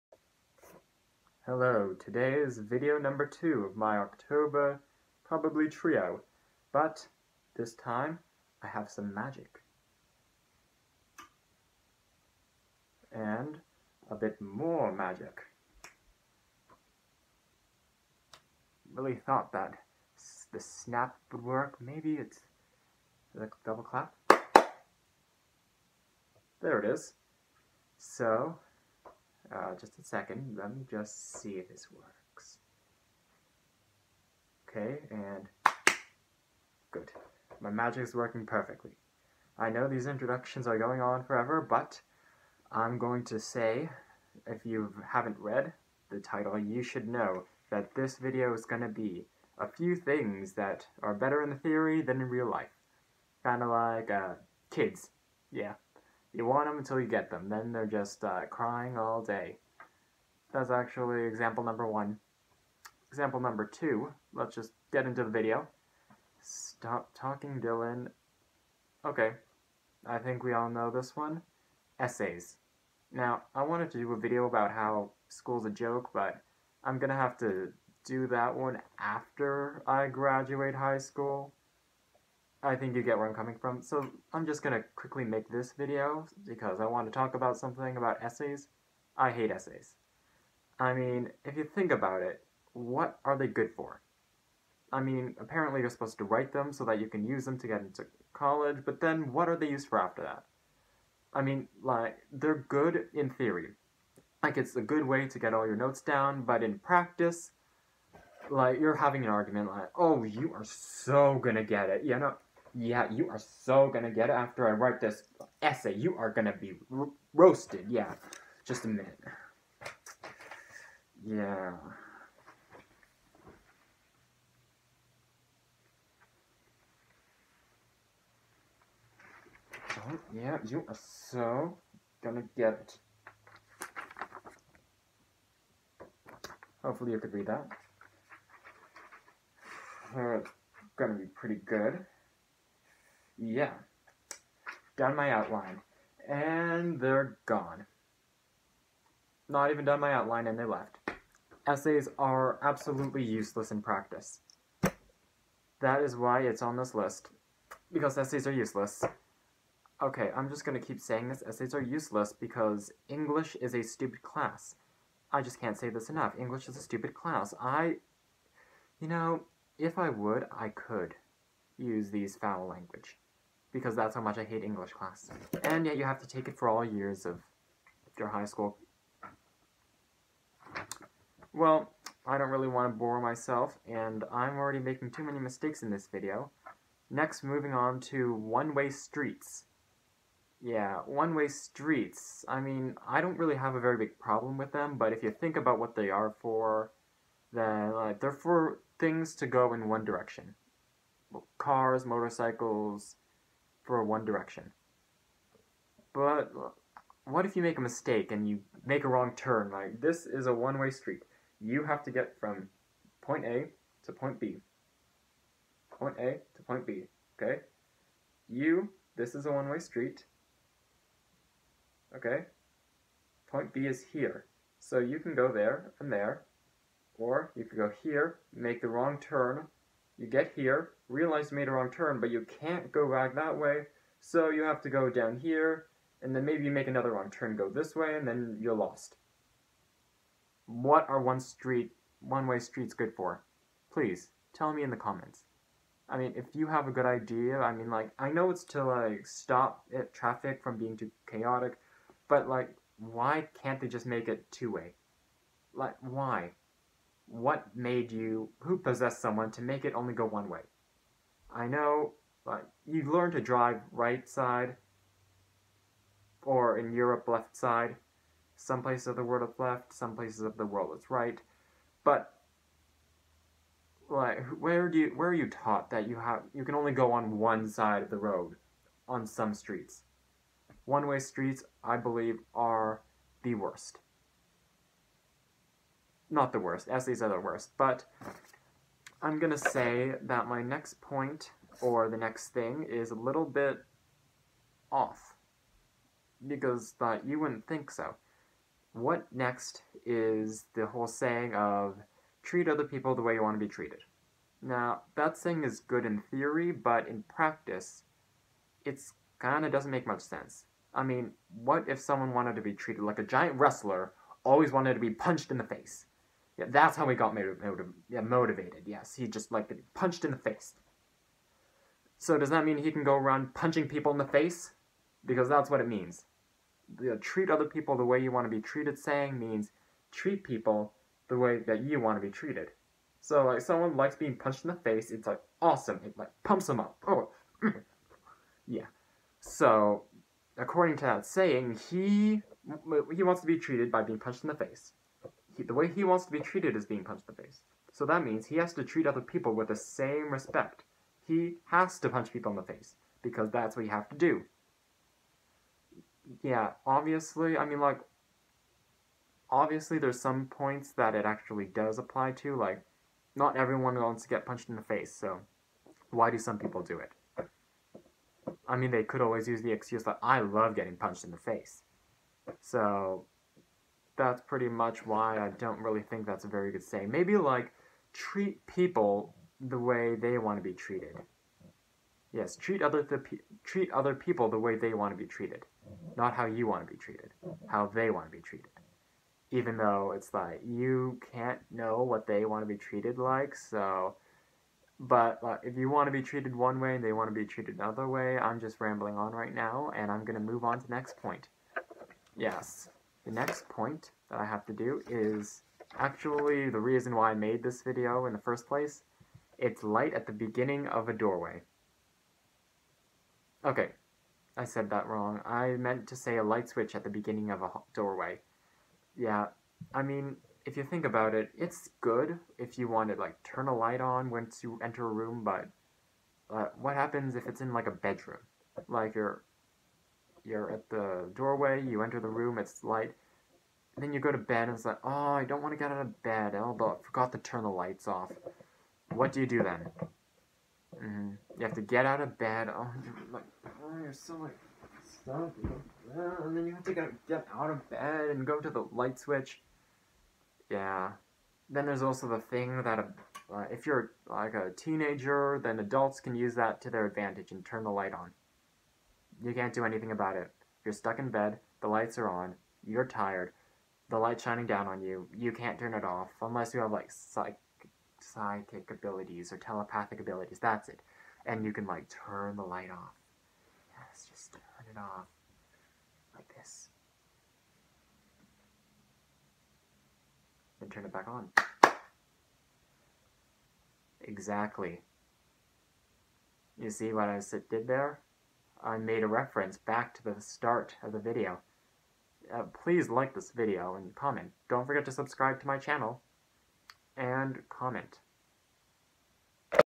Things that are useless in the world
Here are 5 things that seem useful, but after further inspection they are absolutely useless.